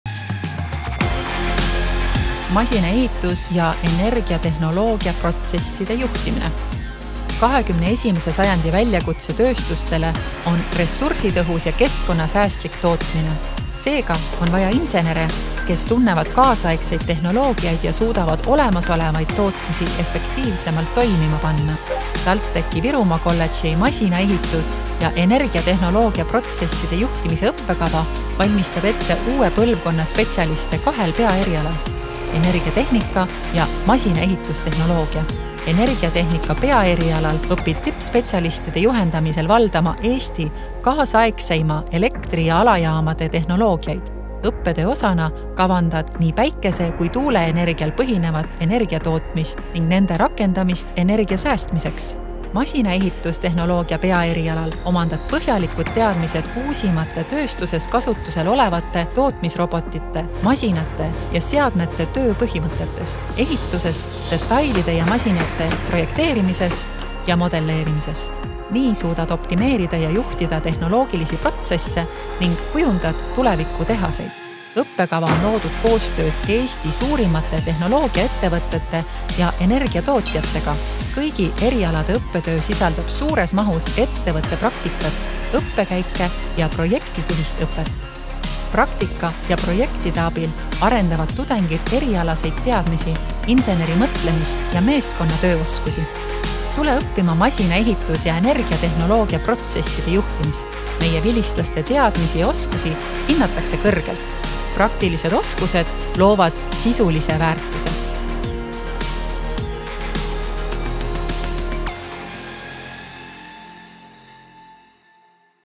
Tule õpi masinaehituse ja energiatehnoloogia protsesside juhtimist! 💡
Masinaehitus- ja energiatehnoloogia protsesside juhtimise nelja-aastane õpe annab sulle laiapõhjalise tehnikahariduse. Kui baasteadmised selged, on sul võimalus valida kahe spetsialiseerumissuuna vahel - masinaehitustehnoloogia või energiatehnika.

Kõik kaks peaeriala on tööturul nõutud ja perspektiivsed valdkonnad, kus palk ületab tunduvalt Eesti keskmist. Lõpetajad leiavad tööd keskastme juhi või spetsialistina masinaehitus-, energeetika- ja mäendusettevõtetes. Samuti on sul piisavalt teadmisi oma ettevõtte rajamiseks.